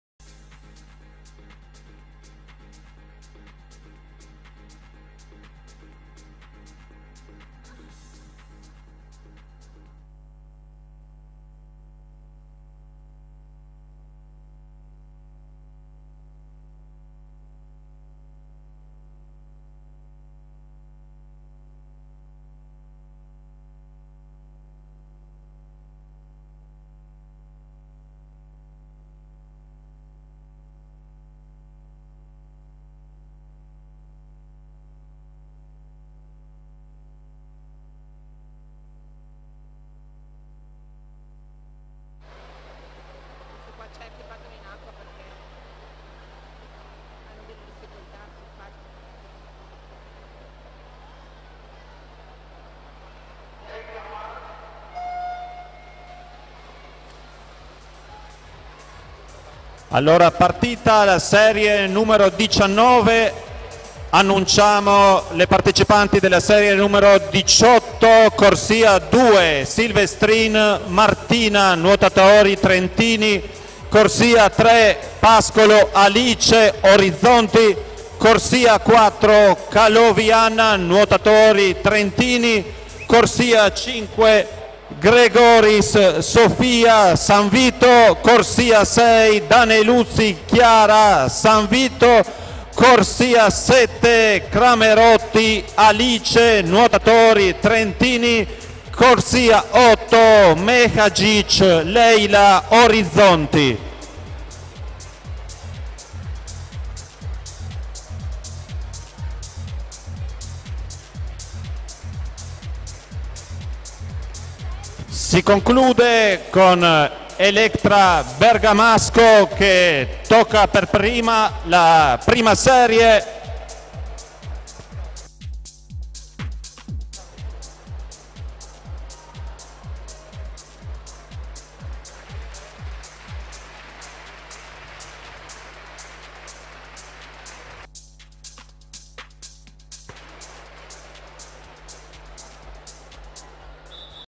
50 Stile Libero Ass. Femminile (Serie 19) - 30 Trofeo Velocita' Soranzio - 18 Memorial Marini
Gara di nuoto svolta a TRIESTE Piscina Bruno Bianchi alle 09:00 di Domenica 19/11/2017

Atleti partecipanti:
in corsia 2 PODREKA PATRIZIA (1973) della soc. Acquamarina Team Trieste ONLUS iscritta con 0:01:11.12
in corsia 3 MERCUSA ALICE (2002) della soc. Acquamarina Team Trieste ONLUS iscritta con 0:01:01.75
in corsia 4 BRUNETTI ROSANNA (1964) della soc. Acquamarina Team Trieste ONLUS iscritta con 0:00:55.00
in corsia 5 DESTRO LORELLA (1971) della soc. ASPEA Padova ONLUS iscritta con 0:00:53.00
in corsia 6 GIACOMINI CLAUDIA (1988) della soc. ASPEA Padova ONLUS iscritta con 0:00:55.00
in corsia 7 BERGAMASCO ELEKTRA (2007) della soc. PALLANUOTO TRIESTE iscritta con 0:01:00.00
in corsia 8 PALAZZI LABINAC LIJLIJANA (1986) della soc. Acquamarina Team Trieste ONLUS iscritta con 0:01:03.00